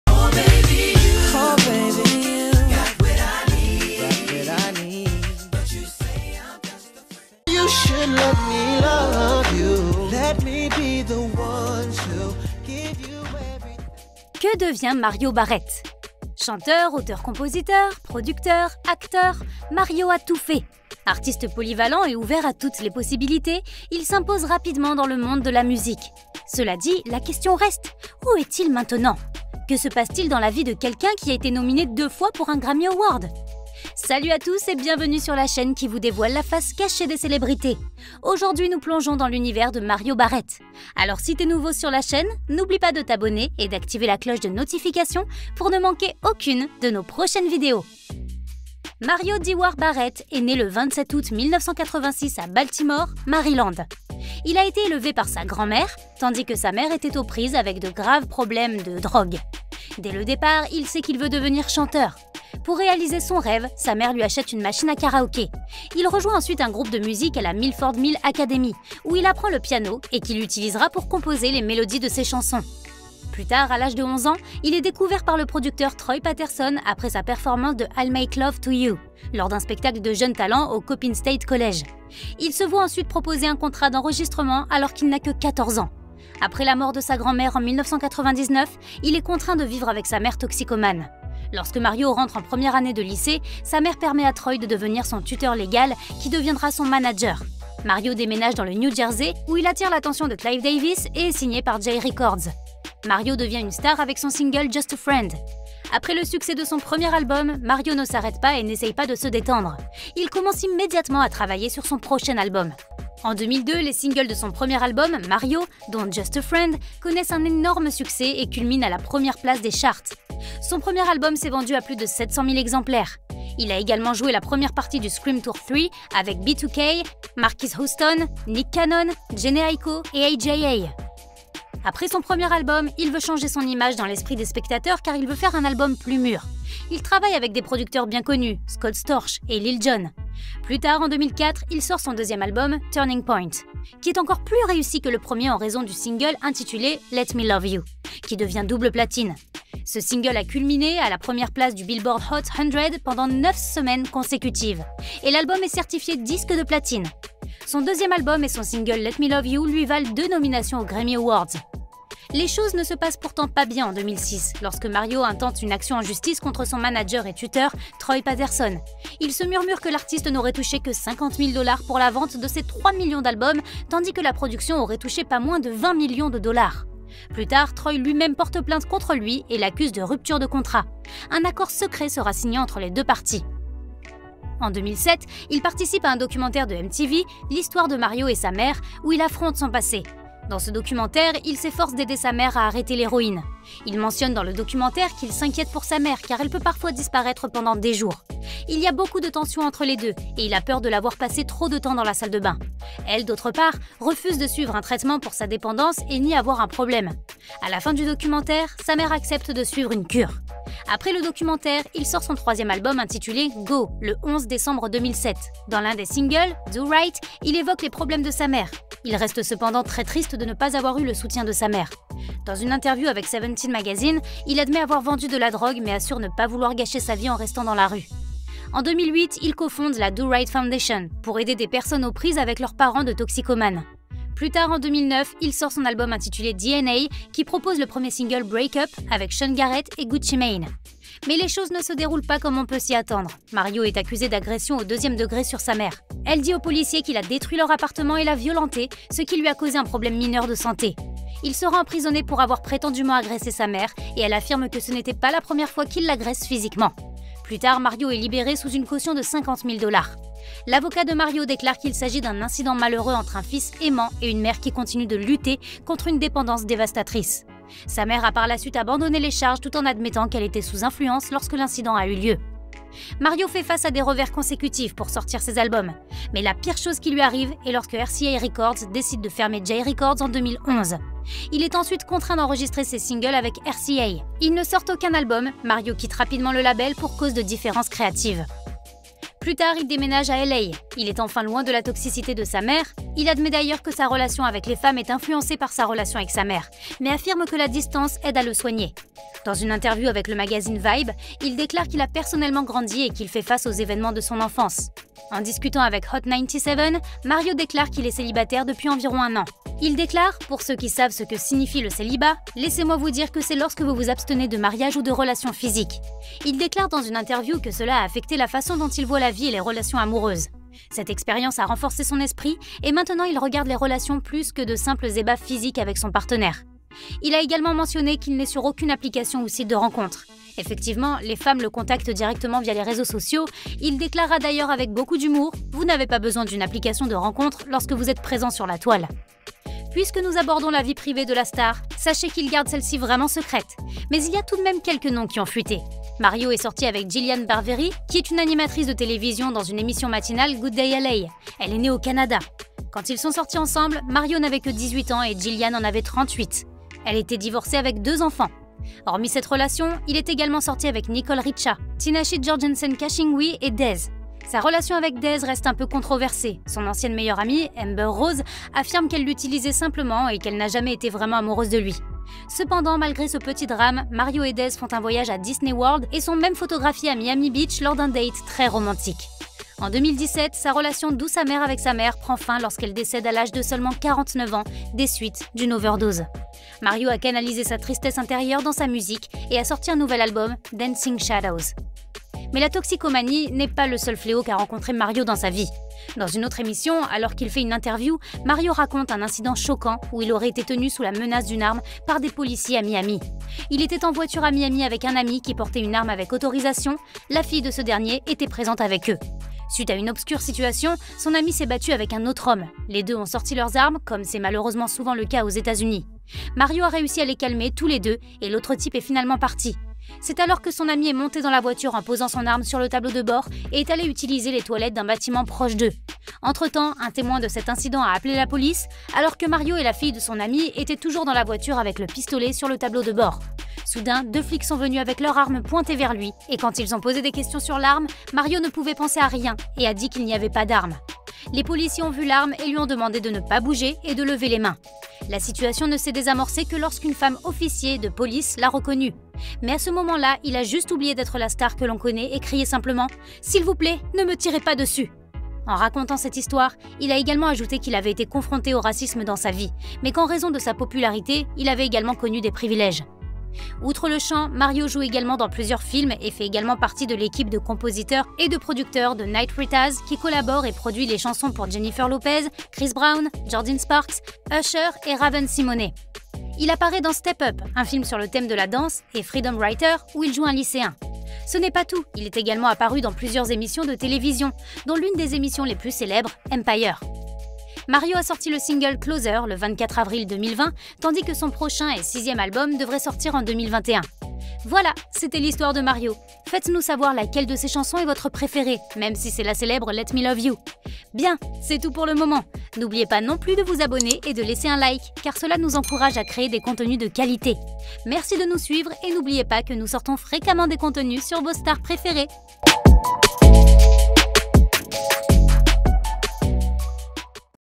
QU'EST-IL ARRIVÉ AU CHANTEUR MARIO ? | Vraies Histoires de Stars
Mario Dewar Barrett (né le 27 août 1986 à Baltimore, Maryland) est un chanteur et acteur de R&B américain.